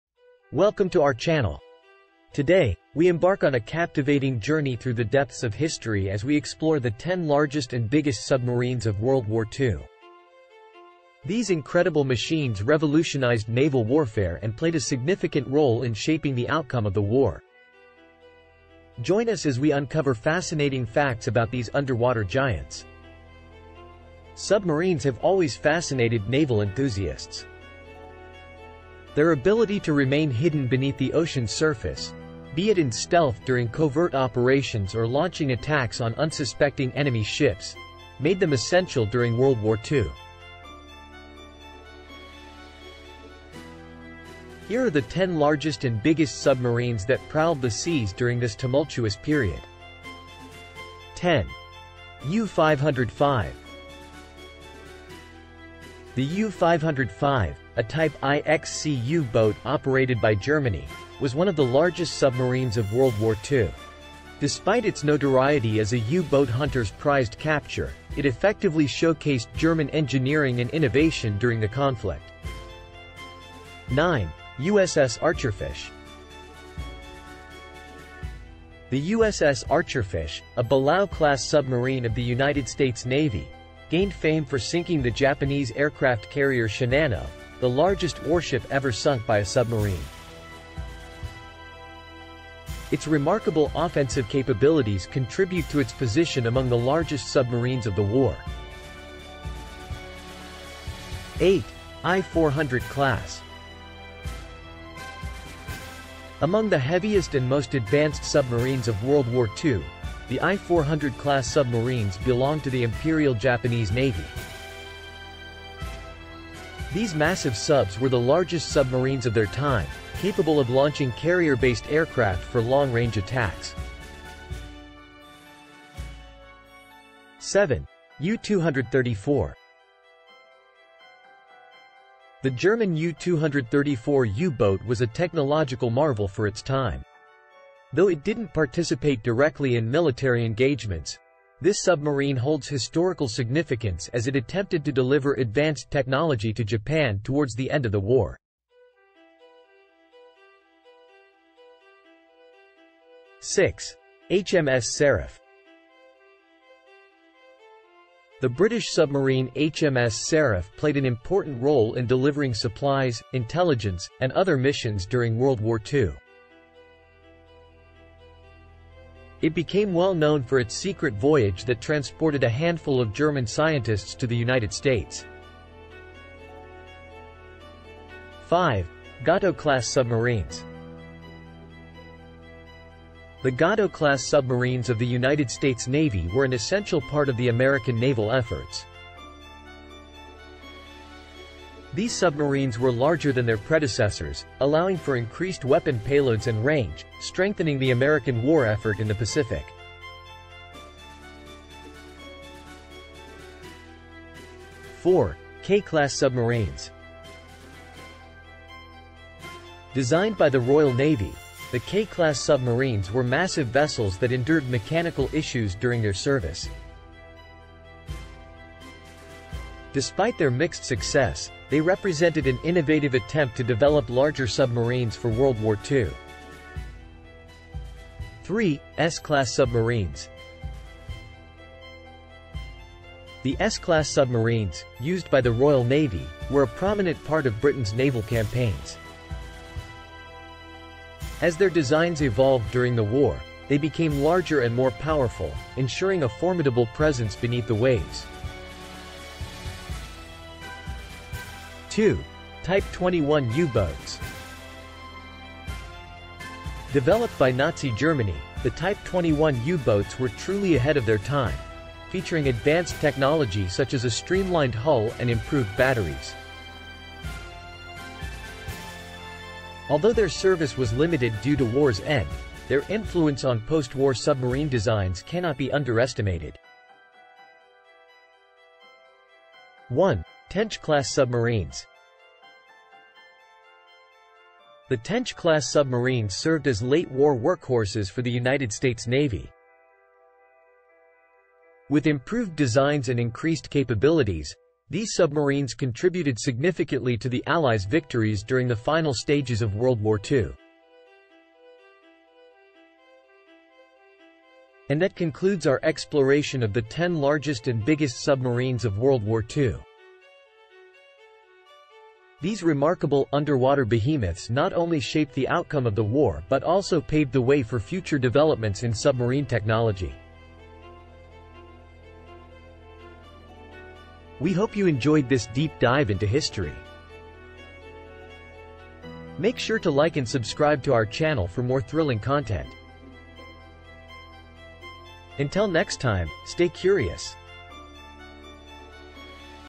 10 Largest and Biggest Submarines of World War II | The Learning loop
In this video, we will explore the top 10 largest and biggest submarines used during World War II. Submarines played a crucial role in naval warfare during the war, providing covert operations and strategic advantages to various countries. Join us as we dive into the history and specifications of these remarkable underwater vessels.